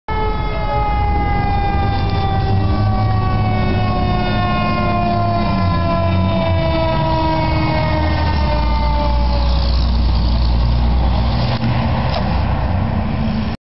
FIRETRUCK!!!
Here's a short video of a firetruck (belonging to Charlottesville Fire Dept.) on West Main Street.